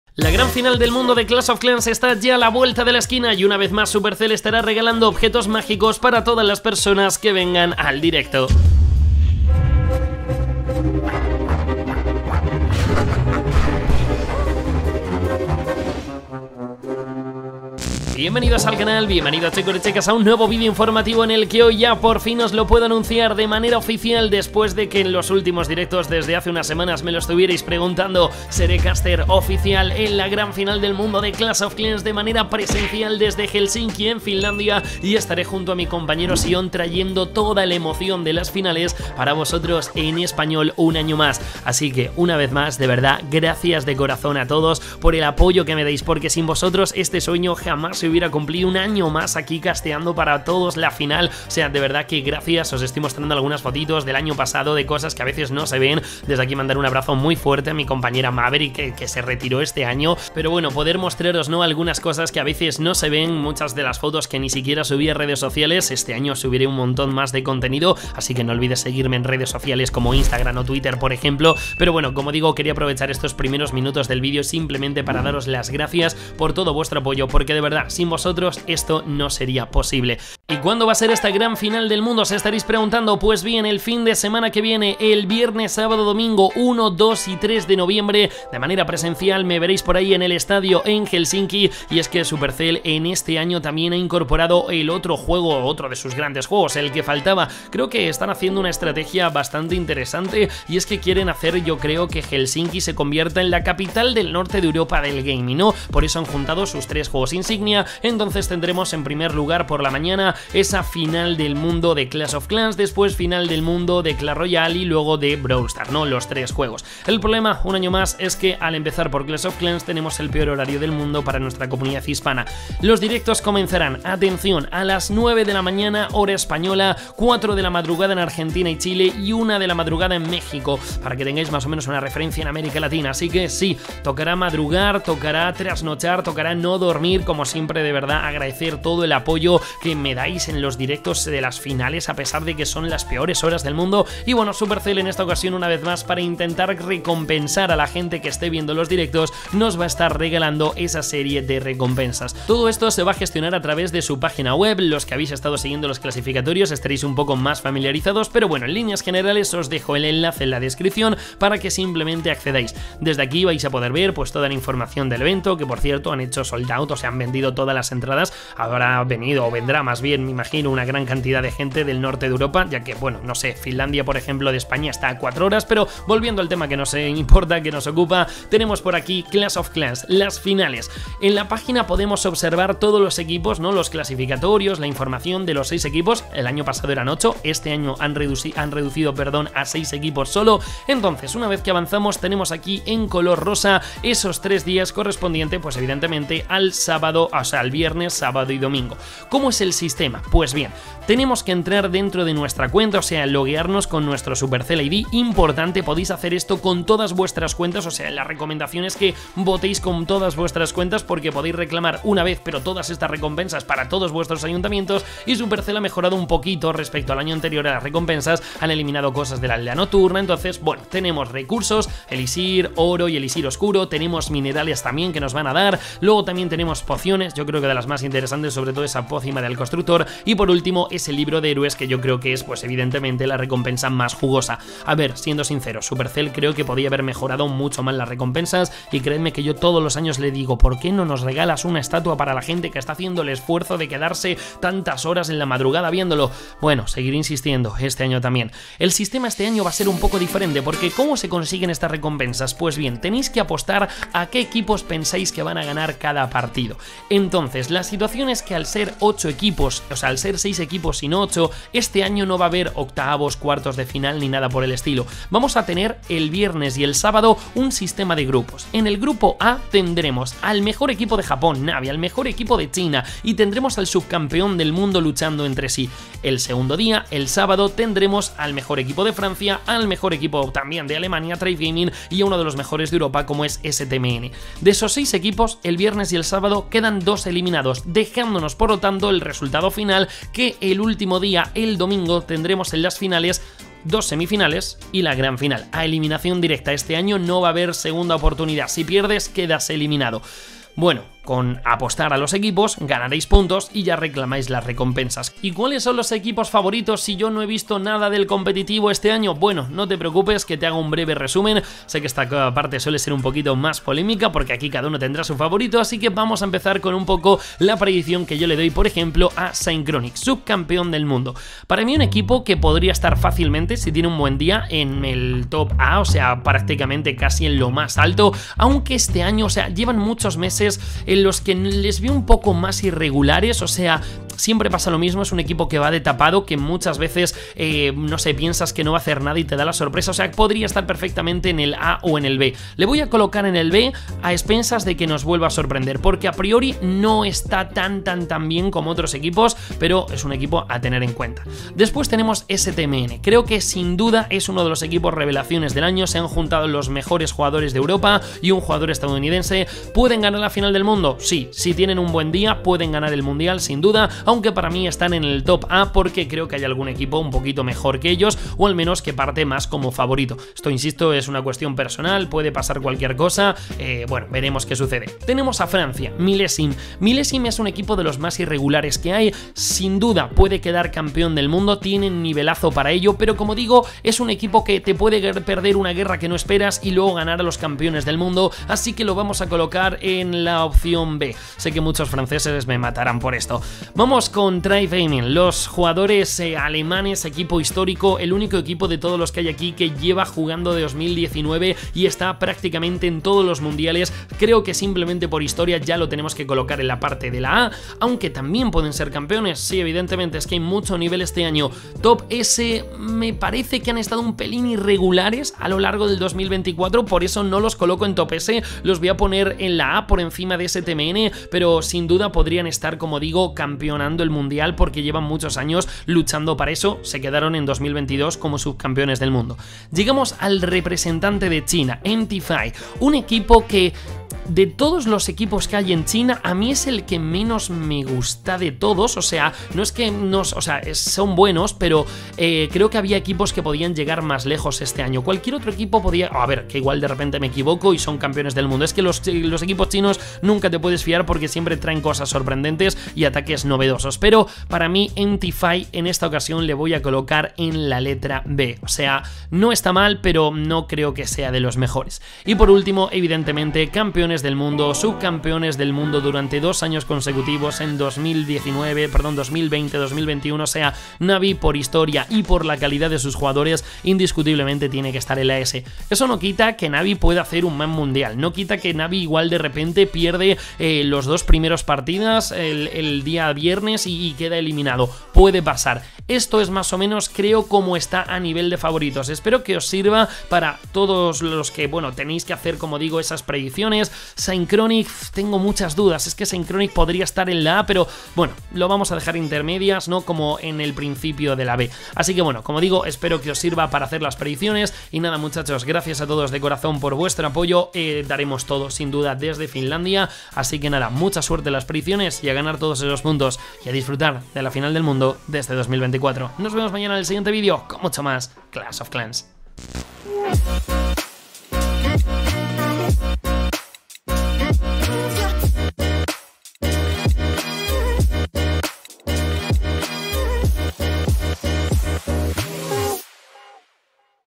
🚨 CÓMO CONSEGUIR *GRATIS* LIBRO de HÉROES, POCIONES y MINERALES! 🚨
Como conseguir gratis las recompensas que este año da supercell con motivo del

📷 INSTAGRAM DEL CANAL: @_zoloko

Capitulos
0:00 Intro
0:27 GRACIAS!
2:25 Recompensas
4:14 Como votar
5:05 Equipos favoritos